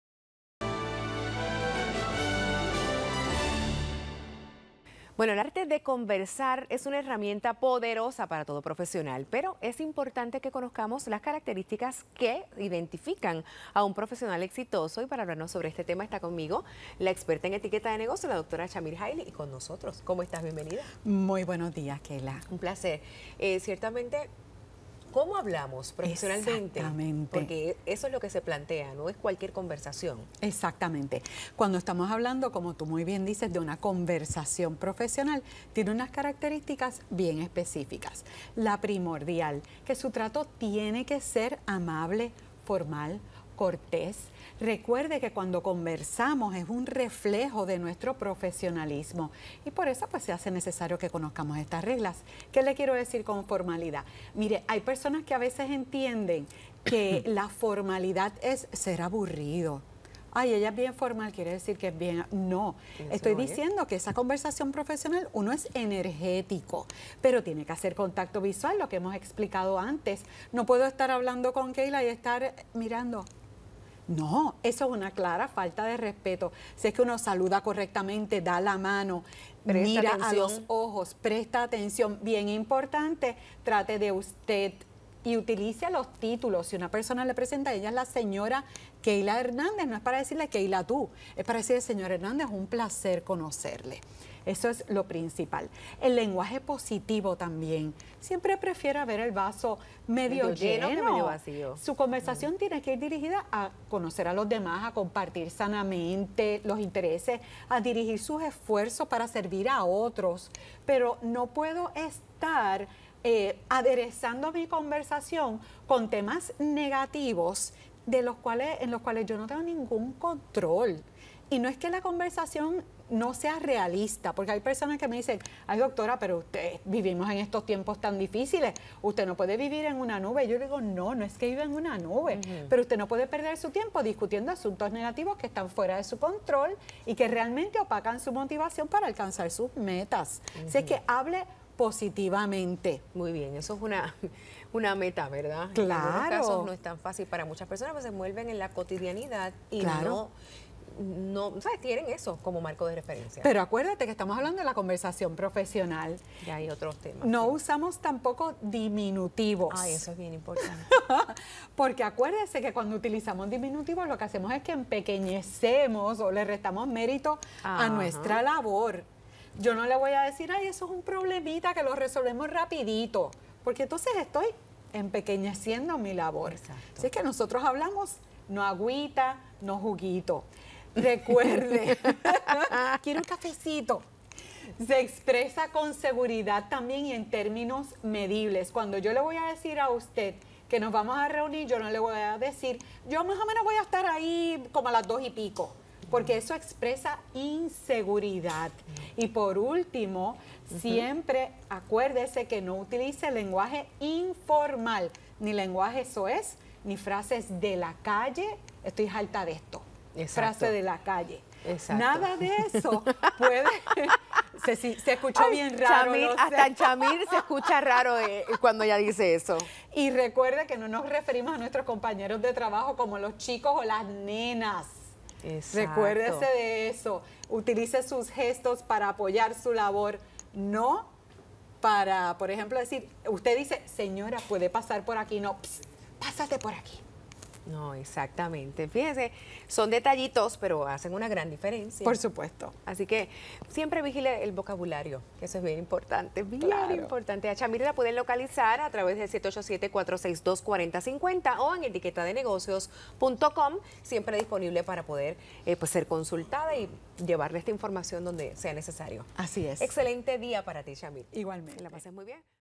Etiqueta de negocios: El Arte de Conversar | WapaTV
La experta de negocios Chamir Highley habla sobre la importancia de saber conversar correctamente.

WapaTV